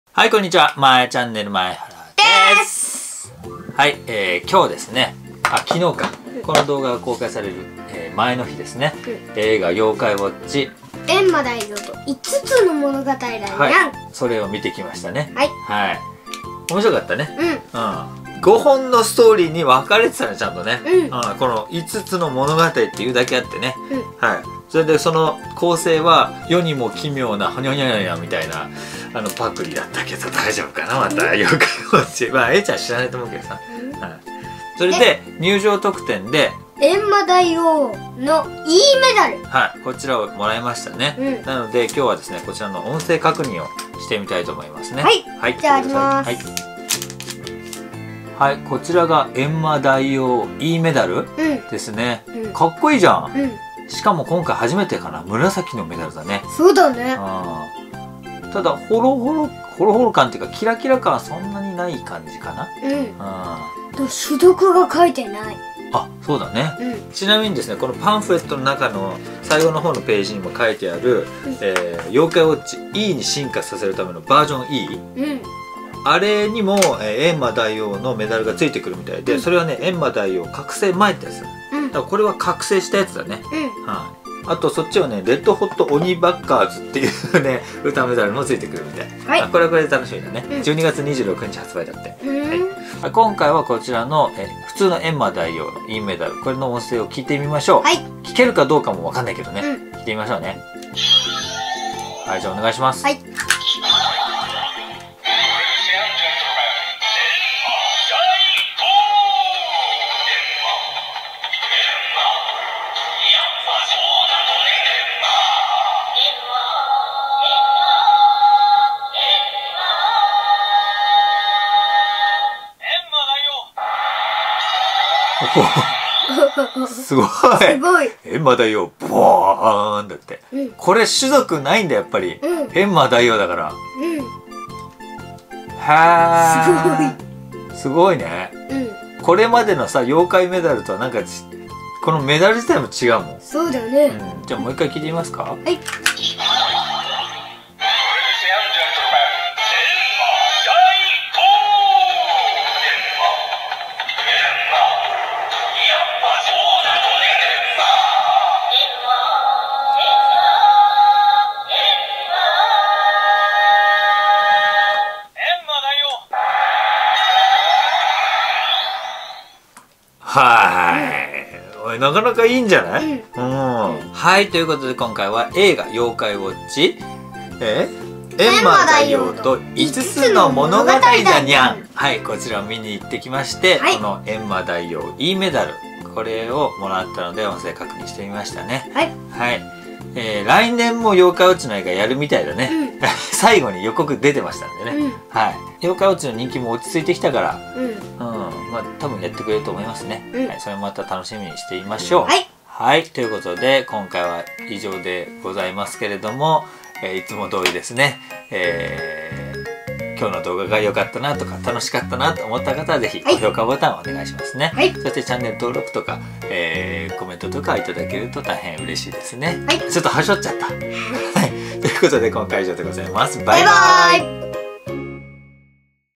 エンマ大王 Eメダル！ 映画 妖怪ウォッチ エンマ大王と5つの物語だニャン！を見てきました。
映画「妖怪ウォッチ エンマ大王と5つの物語だニャン」を見てきました！
タイトル通り5つにお話が分かれており、それぞれ楽しめる内容になっておりました^^

そして今回は来場者特典のエンマ大王 Eメダルを開封、音声確認しております(*^^*)

▽使用音楽（YouTube オーディオ ライブラリ）

▽撮影機材　SONY α NEX-5R（Amazon）